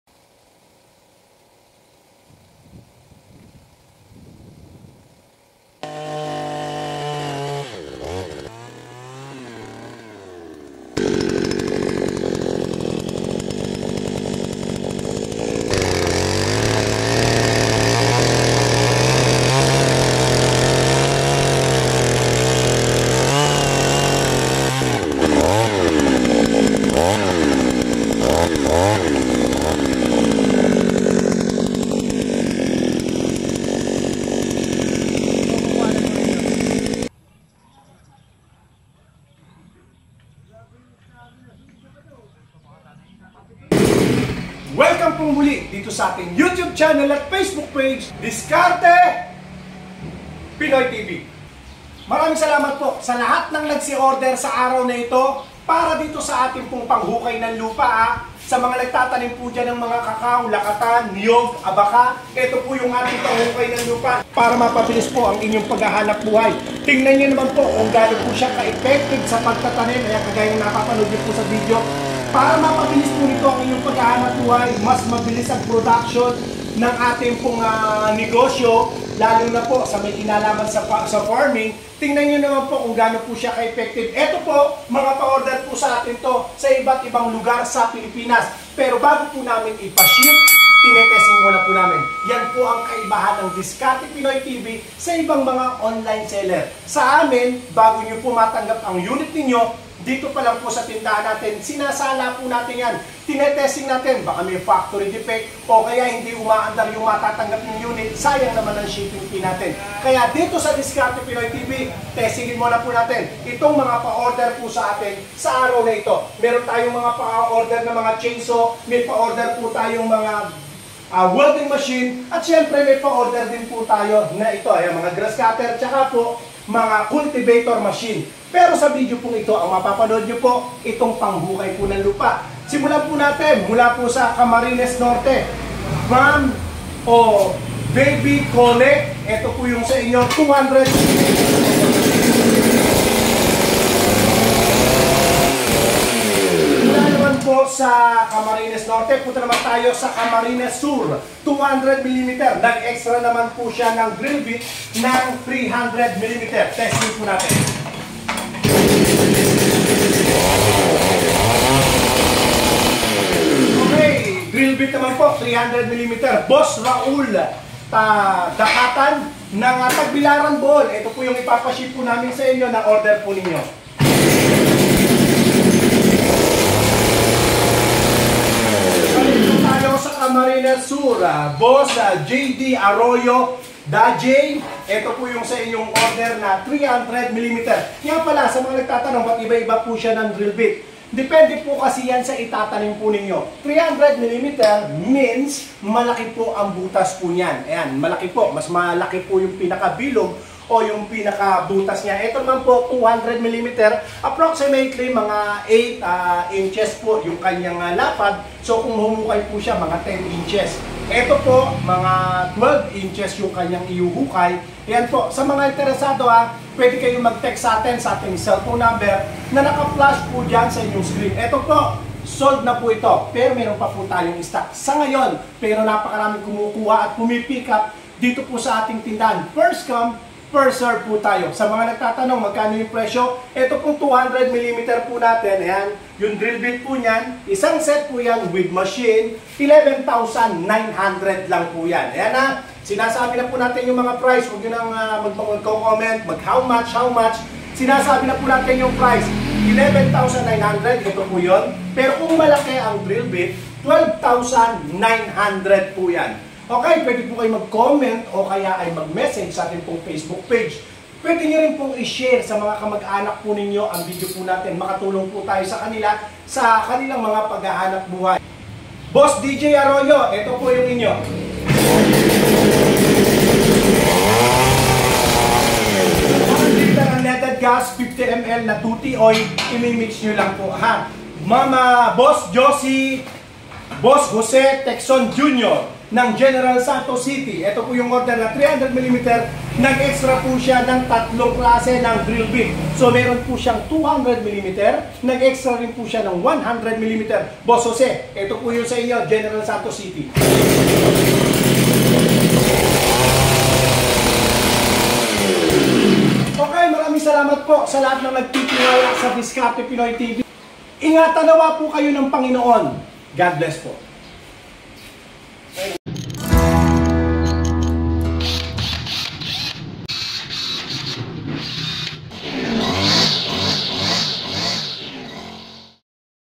ANG LAKAS BUMUTAS, AUGER MACHINE 11,900 ONLY
PARA MABILIS ANG PAGTATANIM NG MGA HALAMAN. AUGER MACHINE ANG KAILANGAN